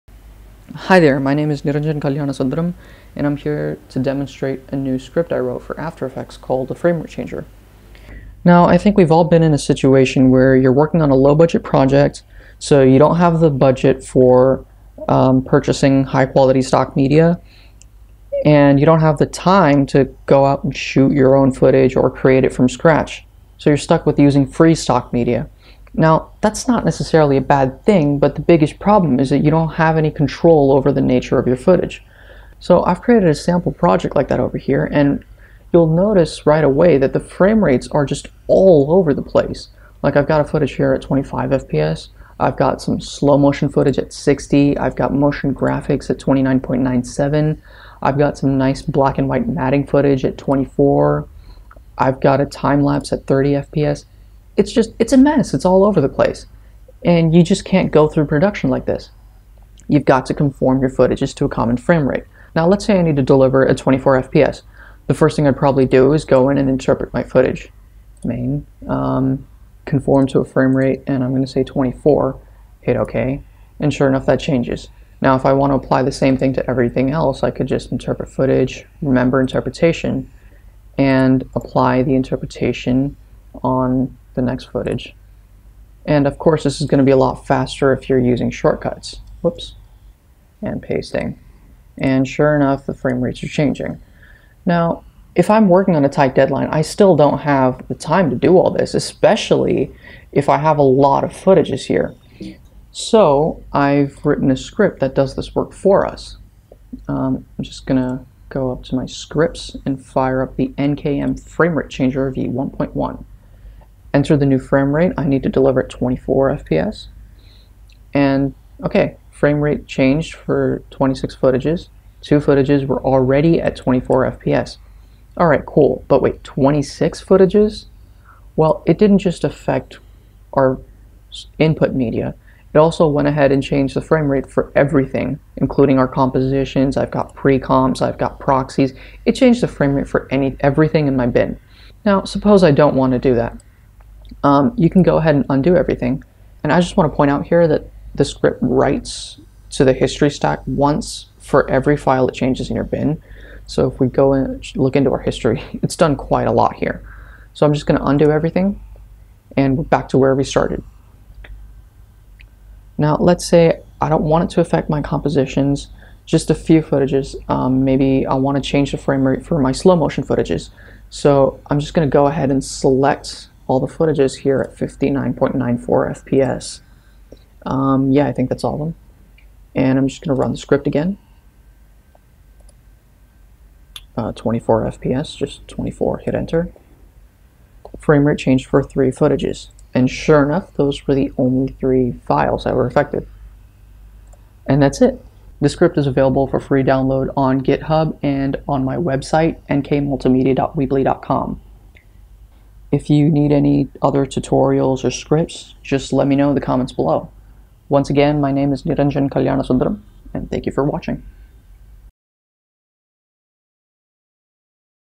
After Effects Framerate Changer Script
This is a demonstration video for an After Effects script that changes the framerate of multiple footages instantly. Download the script here: